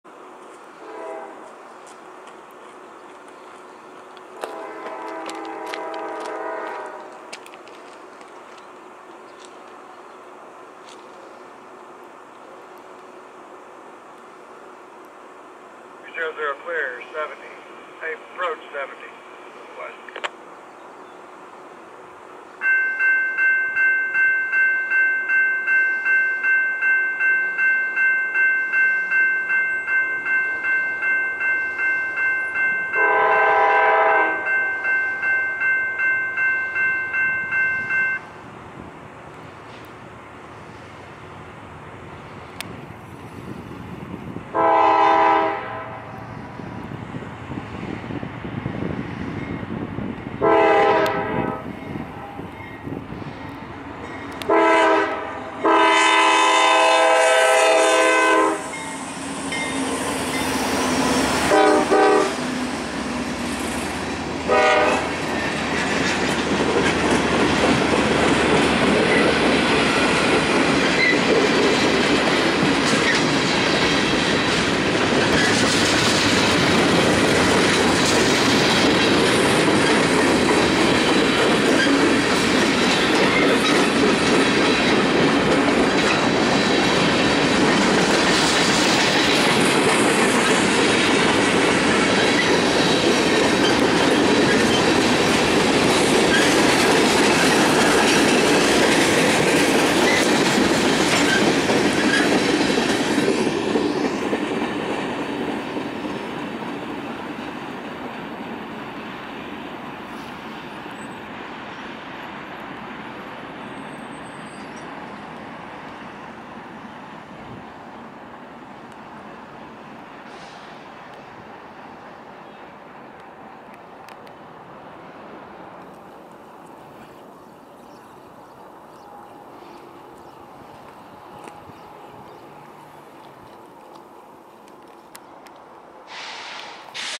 Old Amtrak Boxcar on Norfolk Southern Freight Train
Norfolk Southern local mixed manifest freight train KH00 heads west through the Shawley Drive grade crossing in Hagerstown, Maryland, hauling an old Amtrak boxcar.  Locomotives: CSX 7876(GE C40-8W, built 1993) & CSX 5239(GE ES40DC, built 2005).